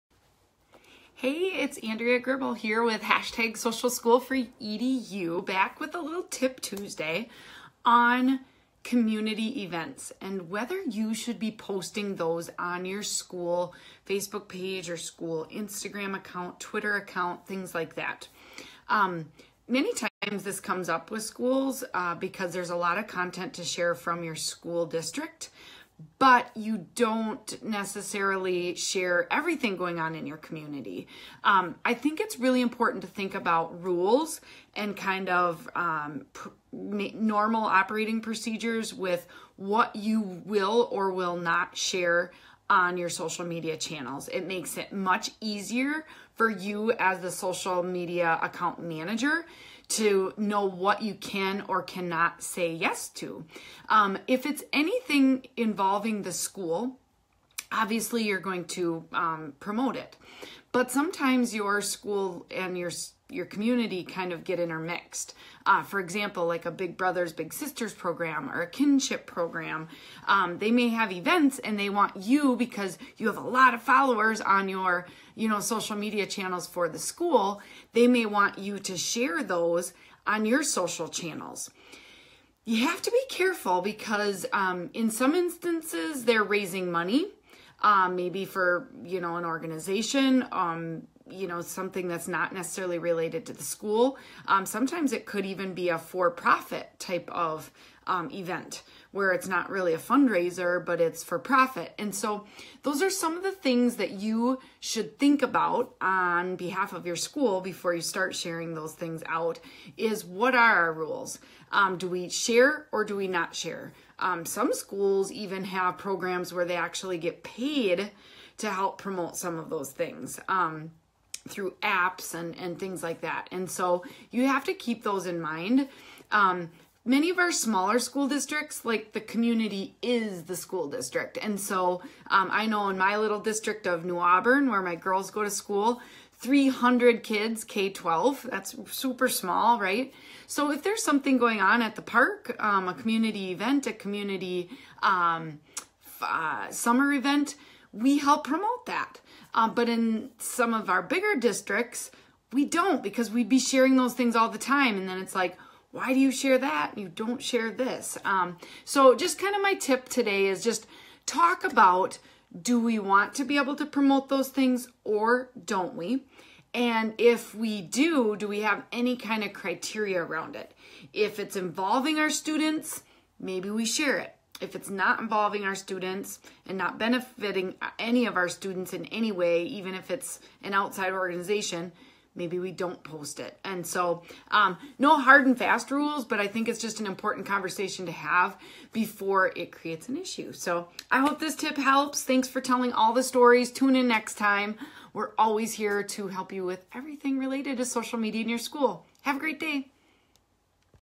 TipTuesday - Community events on your school Facebook page?
Should you post community events on your school's social media accounts?

The answer - it depends... But you want to be consistent!

Never miss updates for your school by signing up for our bi-weekly newsletter